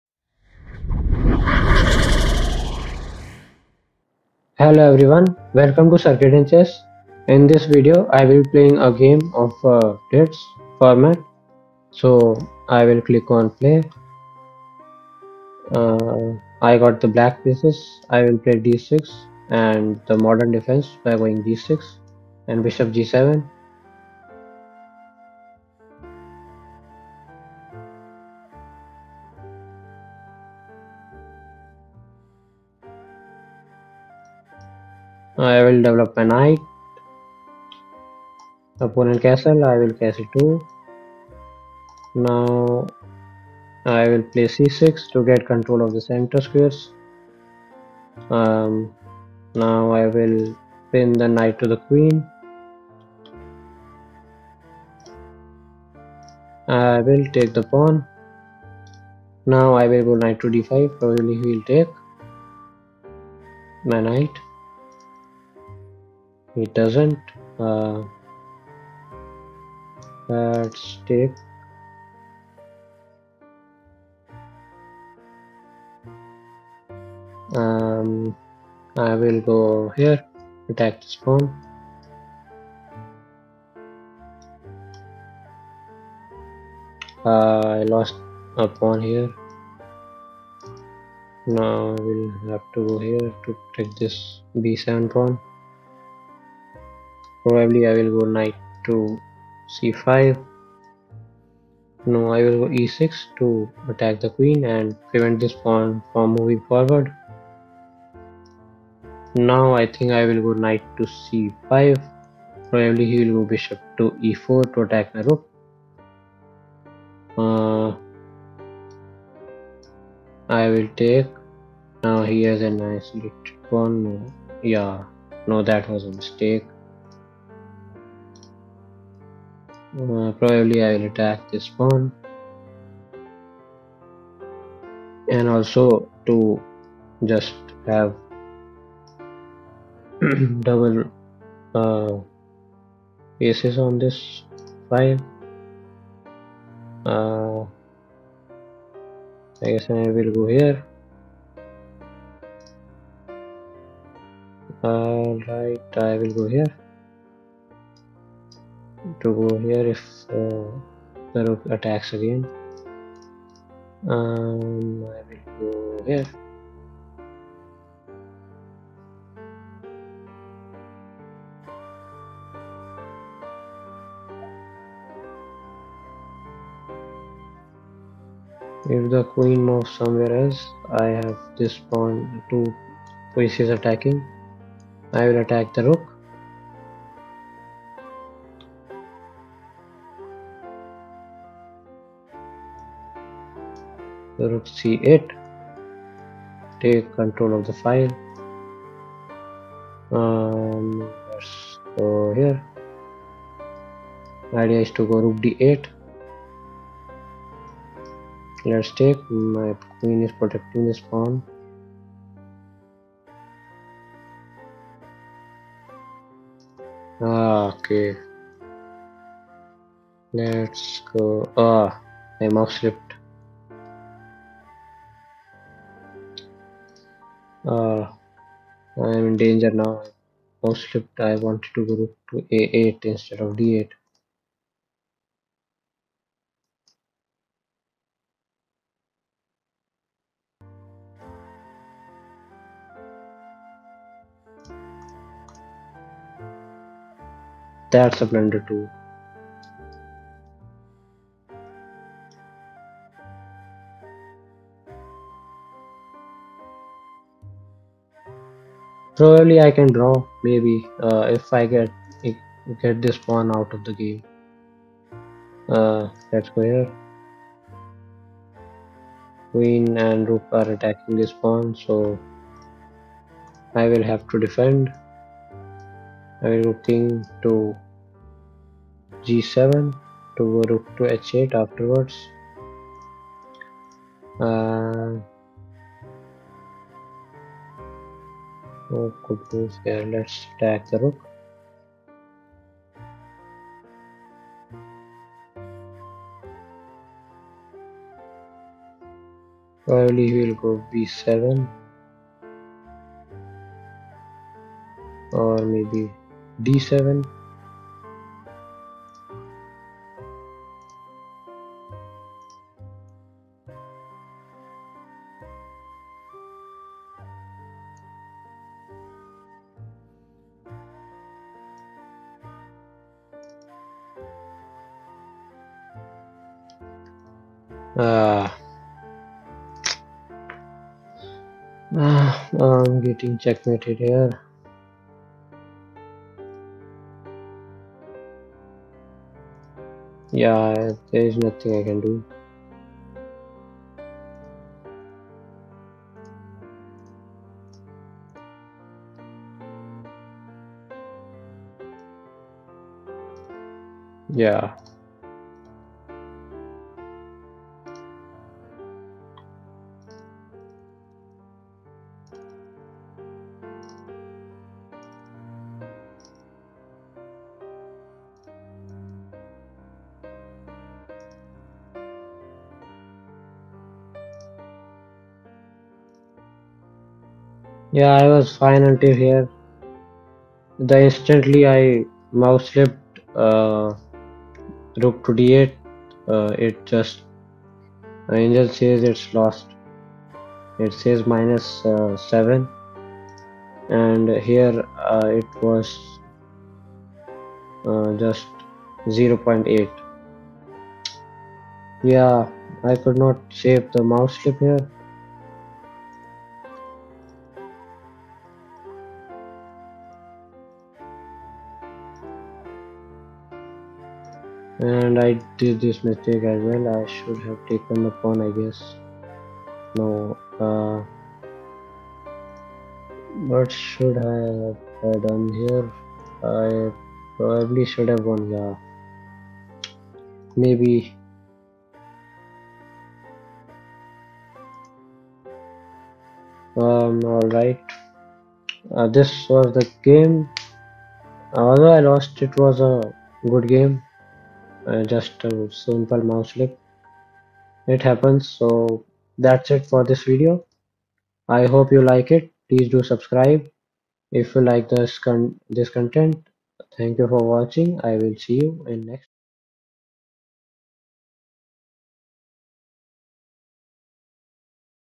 UGH!! THE MOUSESLIP
This game was ruined because of a mouse slip.